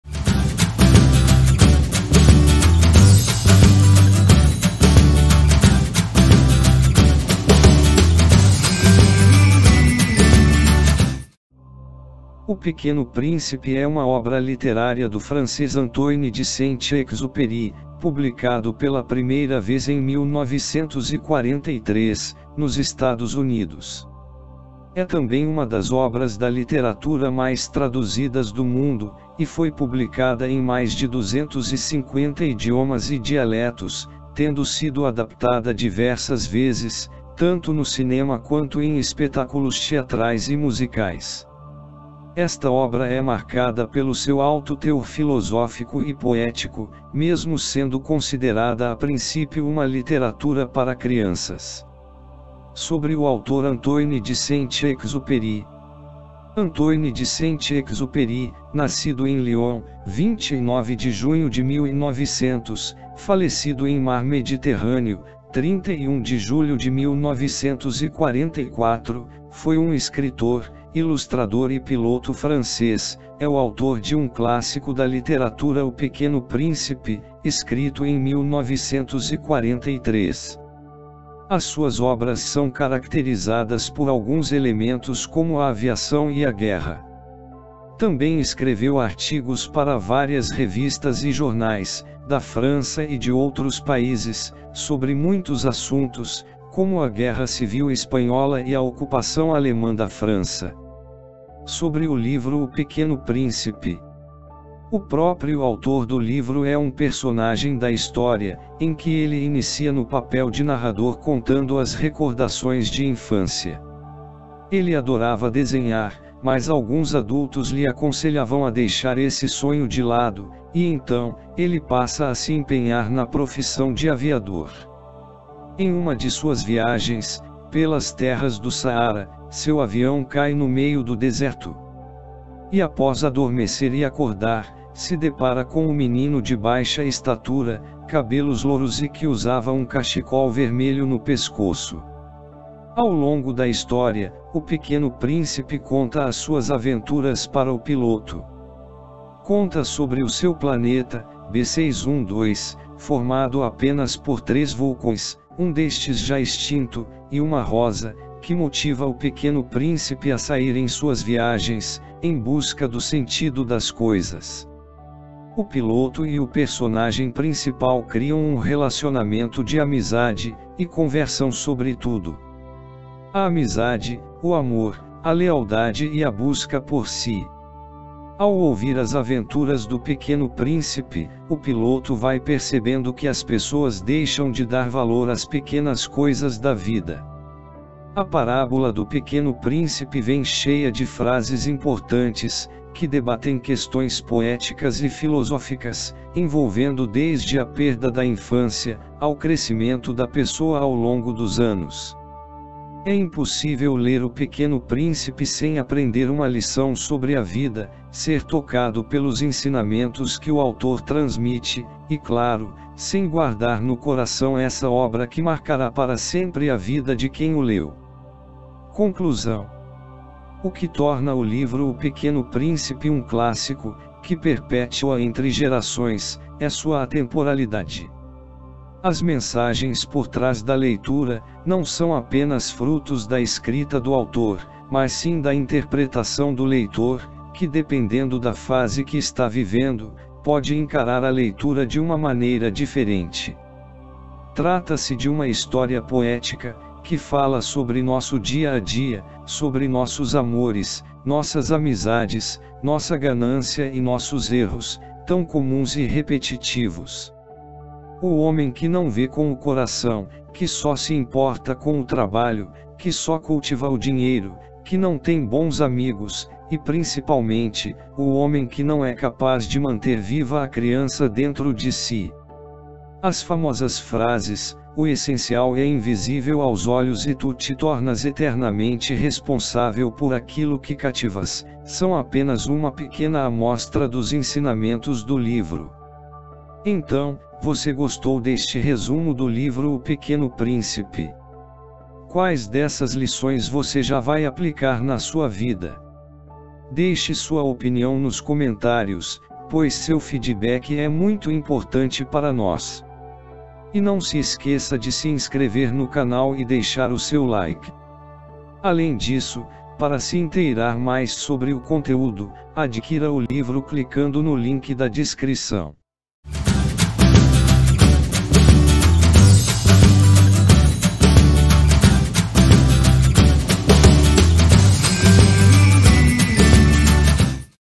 RESUMO DO LIVRO | O PEQUENO PRÍNCIPE | ANTOINE DE SAINT EXUPÉRY
“O Pequeno Príncipe” é uma obra literária do francês Antoine de Saint-Exupéry, publicado pela primeira vez em 1943, nos Estados Unidos. É também uma das obras da literatura mais traduzidas do mundo, e foi publicada em mais de duzentos e cinquenta idiomas e dialetos, tendo sido adaptada diversas vezes, tanto no cinema quanto em espetáculos teatrais e musicais.
Esta obra é marcada pelo seu alto teor filosófico e poético, mesmo sendo considerada a princípio uma literatura para crianças.
O próprio autor do livro é um personagem da história, em que ele inicia no papel de narrador contando as recordações de infância. Ele adorava desenhar, mas alguns adultos lhe aconselhavam a deixar esse sonho de lado, e então, ele passa a se empenhar na profissão de aviador.
Em uma de suas viagens, pelas terras do Saara, seu avião cai no meio do deserto. E após adormecer e acordar, se depara com o menino de baixa estatura, cabelos louros e que usava um cachecol vermelho no pescoço.
Ao longo da história, o Pequeno Príncipe conta as suas aventuras para o Piloto.